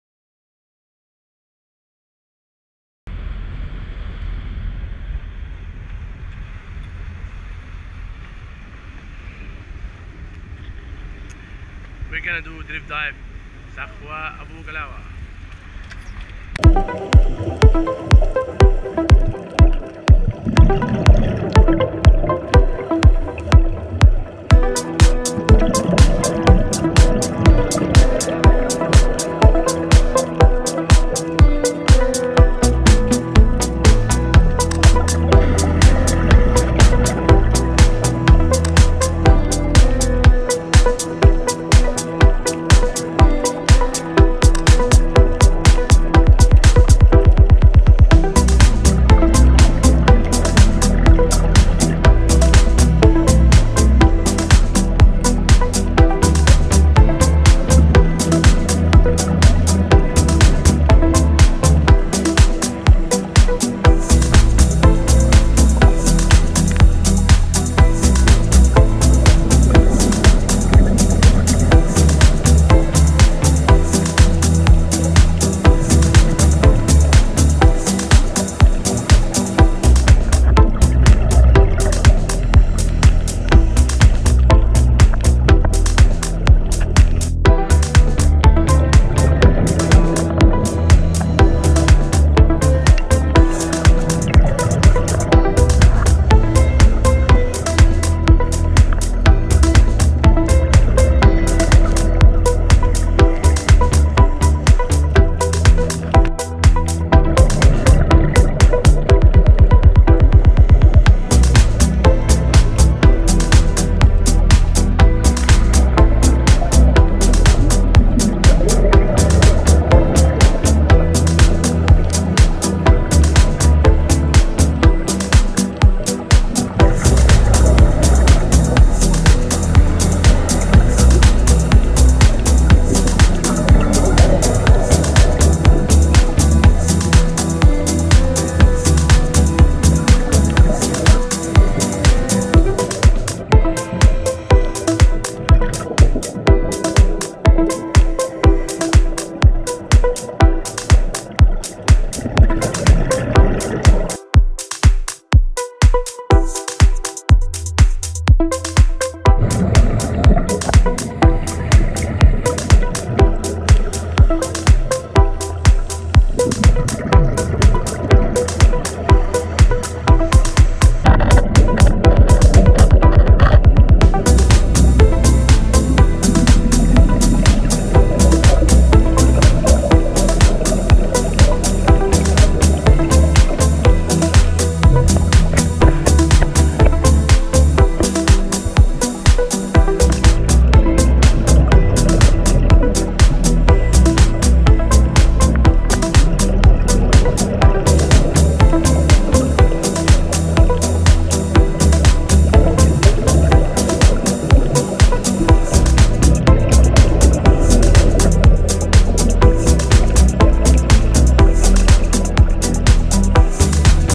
Drift Dive Sakhwat Abu Glawa Hurghada Egypt Red Sea
An Excellent Dive spot for all Divers Levels and beginners,
Its characterized by the diversity of fishes that live in it,
About 45 minutes from the tourist port North Hurghada.